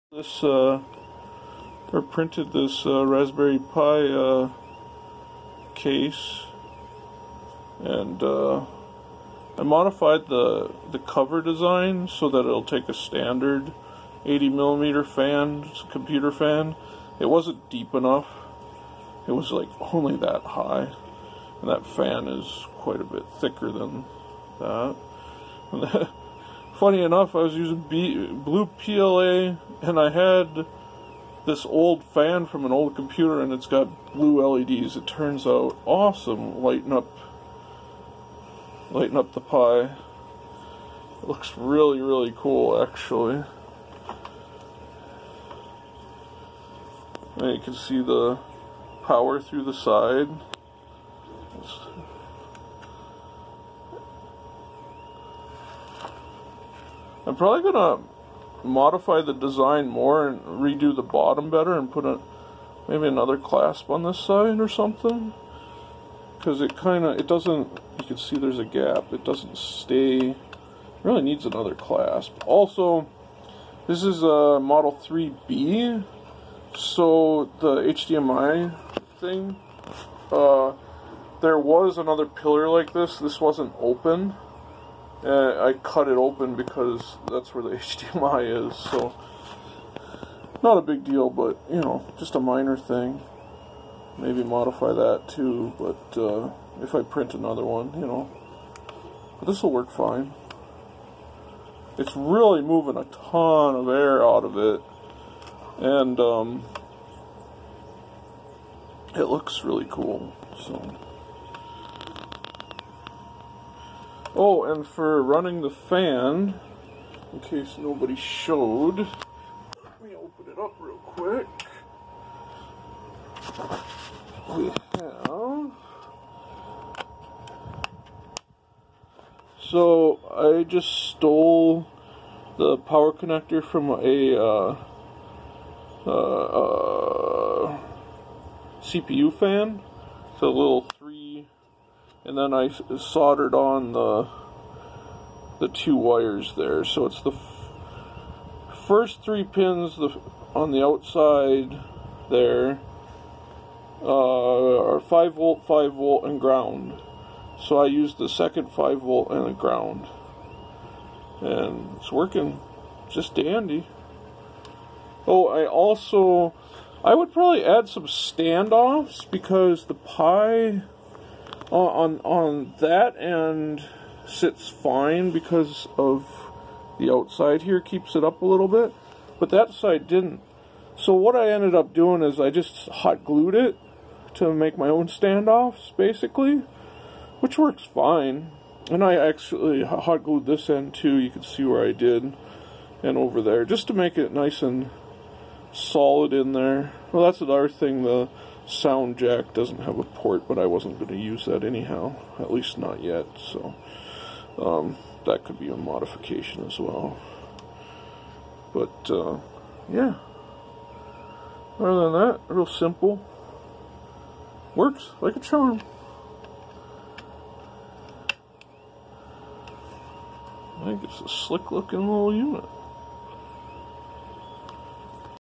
3d printed 80mm fan Raspberry Pi case
Just an overview of a 3d printed 80mm computer fan case I modified for my Raspberry Pi you can download the stl files for the most recent version